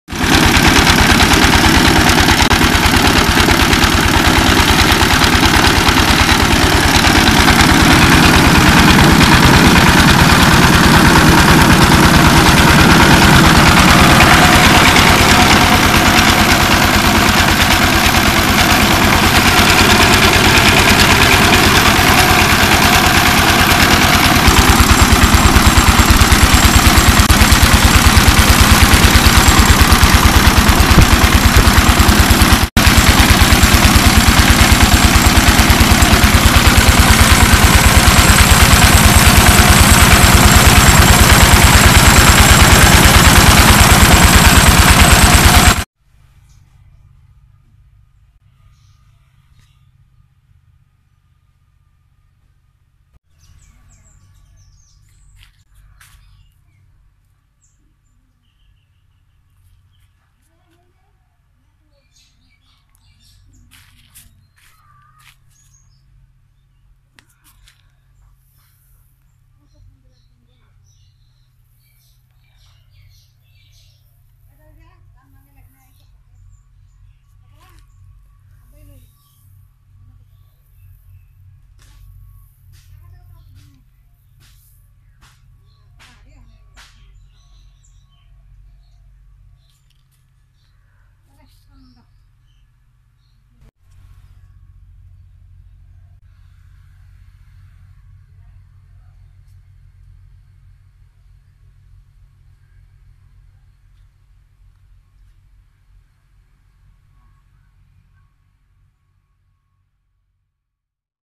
Traditional process of rice Husking- Bangladesh.
A rice huller or rice husker is an agricultural machine used to automate the process of removing the chaff (the outer husks) of grains of rice. Throughout history, there have been numerous techniques to hull rice. Traditionally, it would be pounded using some form of mortar and pestle. An early simple machine to do this is a rice pounder. Later even more efficient machinery was developed to hull and polish rice. These machines are most widely developed and used throughout Asia where the most popular type is the Engelberg huller designed by German Brazilian engineer Evaristo Conrado Engelberg in Brazil and first patented in 1885.